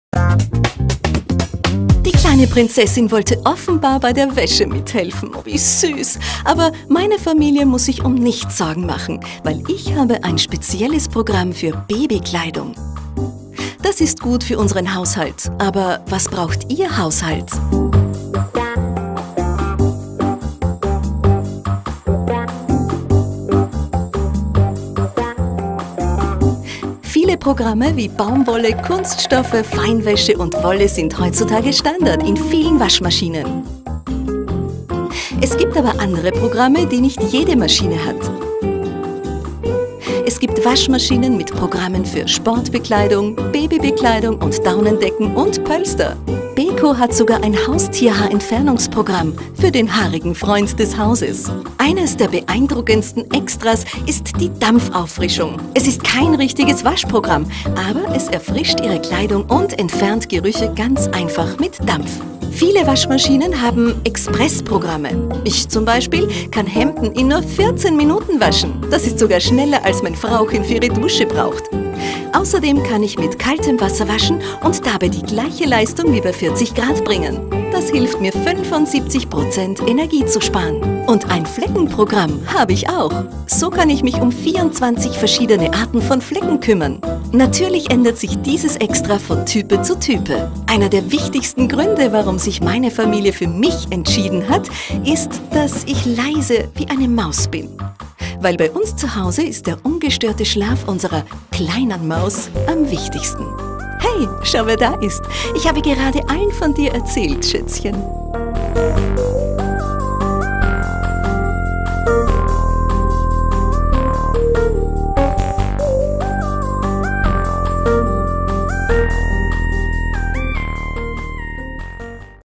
Kauf einer Waschmaschinen
Beko zeigt euch, was ihr beim Kauf einer neuen Waschmaschine beachten sollte, vor allem auf welche Programme ihr Wert legen solltet.